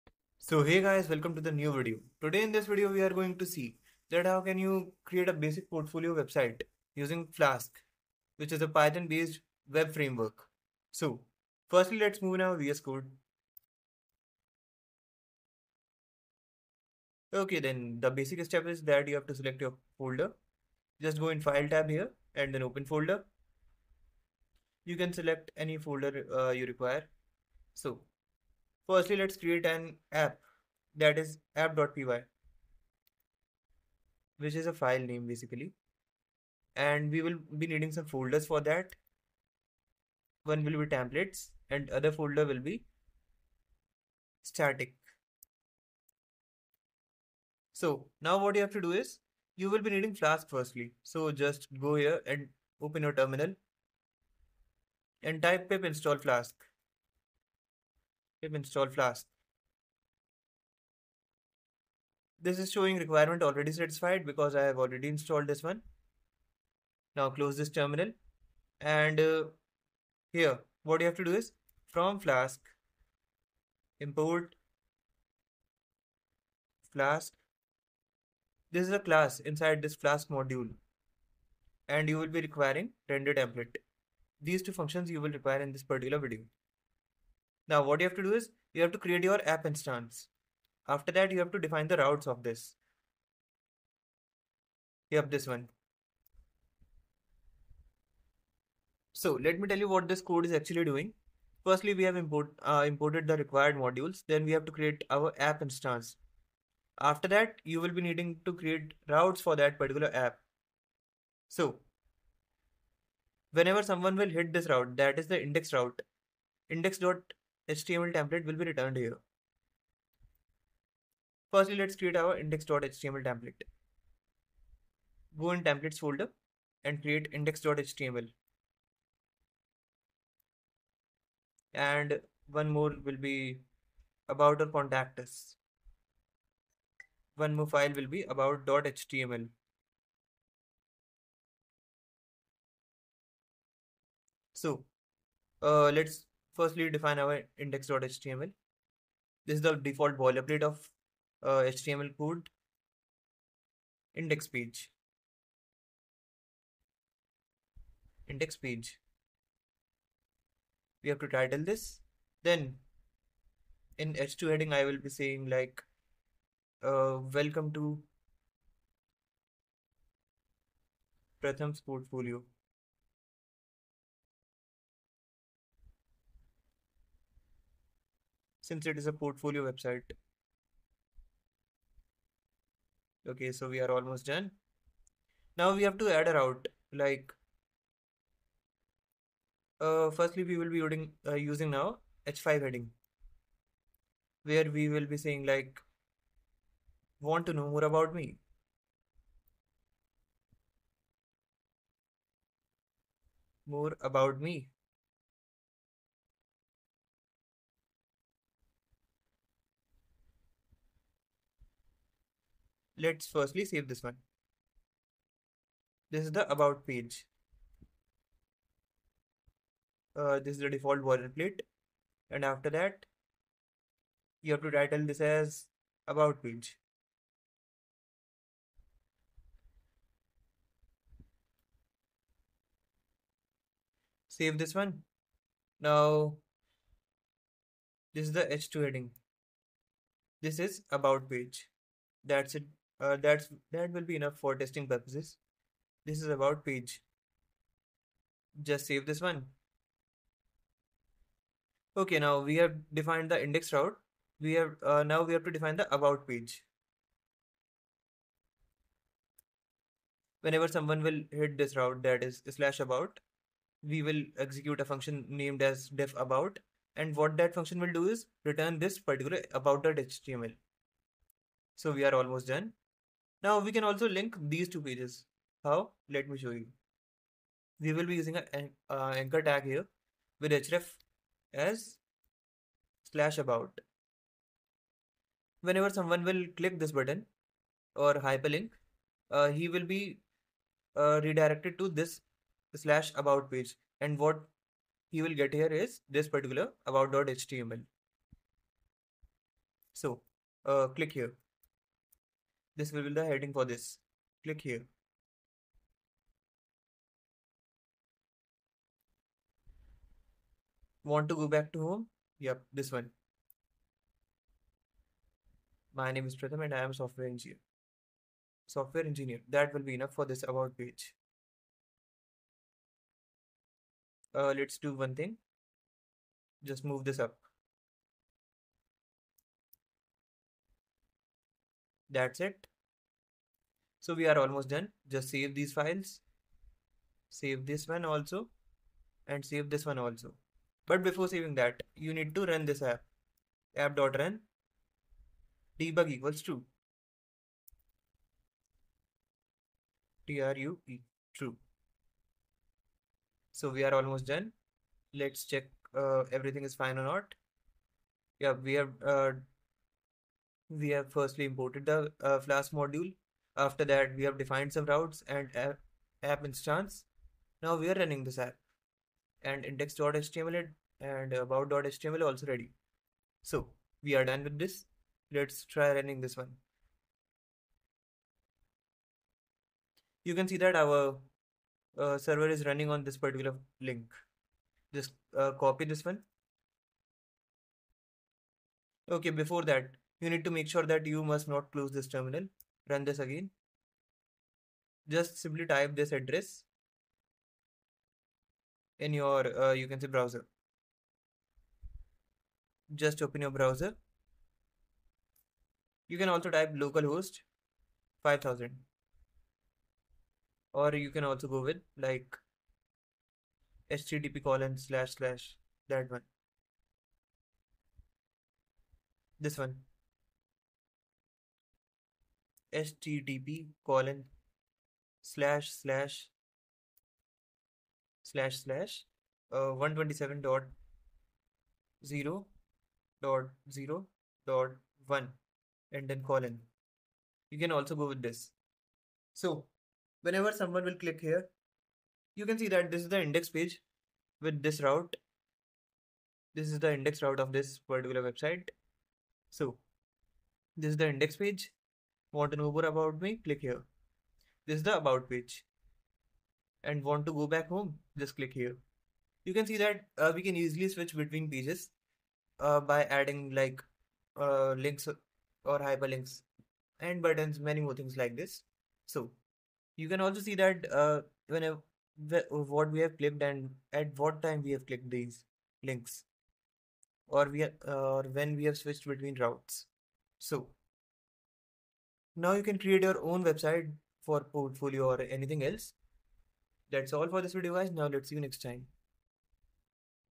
How to Build a Portfolio Website with Flask | Personal Website Tutorial for Python Web Developers
Want to showcase your skills and projects online? 🌟 In this beginner-friendly tutorial, you’ll learn how to build a personal portfolio website with Flask, a lightweight and powerful Python web framework.

This project is perfect for anyone looking to:

Build their personal brand

Add real-world web dev projects to their resume

Learn the basics of Flask, HTML/CSS, and templating

🧠 In this video, you’ll learn:

How to install Flask and set up your project

Create routes for your home, about, and projects pages

Use Jinja2 templating for dynamic content

Add contact forms and project showcases

Bonus: Deploy your portfolio online using GitHub Pages, Render, or PythonAnywhere

✅ Great for:
✔️ Students
✔️ Developers building their first website
✔️ Freelancers
✔️ Anyone looking to learn backend + frontend integration with Python

🎯 Continue your learning with our Python Flask Project Series, including blog apps, login systems, and full-stack tools!